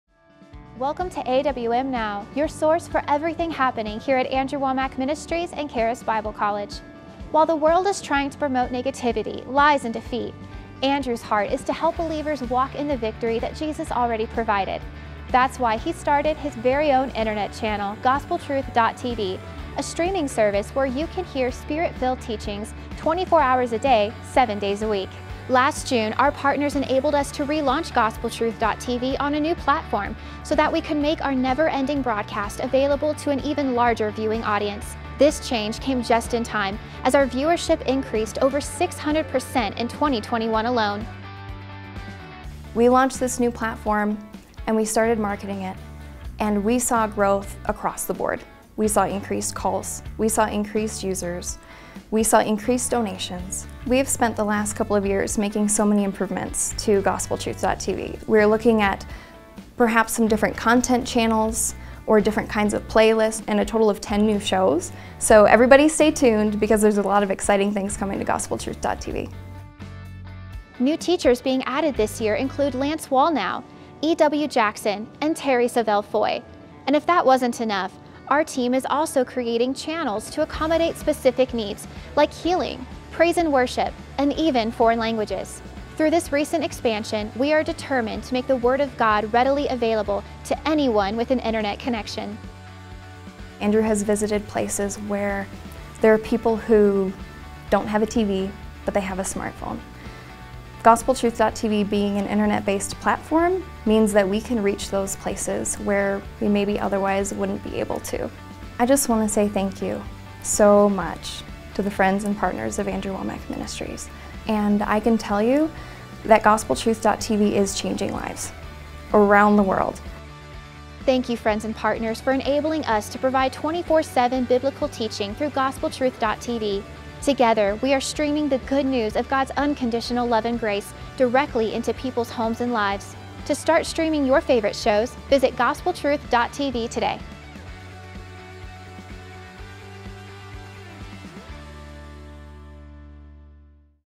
AWM Now: Expanding Andrew’s Internet Channel for You
While the world is trying to promote negativity, lies and defeat, Andrew’s heart is to help believers walk in the victory Jesus has already provided. That’s why he created his very own internet channel, GospelTruth.tv, a streaming service where you can hear spirit-filled teachings twenty-four hours a day, seven days a week. Last June, our partners enabled us to relaunch GospelTruth.TV on a new platform, with more features than ever before!